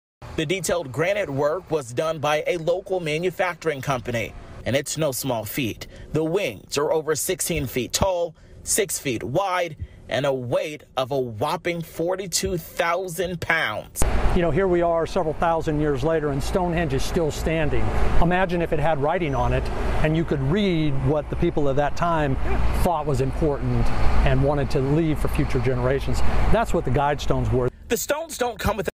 Bizarre way to describe the message on the Georgia Guidestones
This is one of the most bizarre 30 seconds of video from the past year. Listen to what the guy in the yellow shirt says at the end.

"Read what the people of that time thought was important and wanted to leave for future generations."

Huh...?

Here's what the Guidestones said, in 8 different languages:

1. Maintain humanity under 500,000,000 in perpetual balance with nature.

2. Guide reproduction wisely — improving fitness and diversity.

3. Unite humanity with a living new language.

4. Rule Passion — Faith — Tradition — and all things with tempered reason.

5. Protect people and nations with fair laws and just courts.

6. Let all nations rule internally resolving external disputes in a world court.

7. Avoid petty laws and useless officials.

8. Balance personal rights with social duties.

9. Prize truth — beauty — love — seeking harmony with the infinite.

10. Be not a cancer on the earth — Leave room for nature — Leave room for nature.

Well, maybe it is what "we" thought was important.

According to Wired, the translations (into 8 languages) were provided by the United Nations.  Not sure where Wired go that info, though.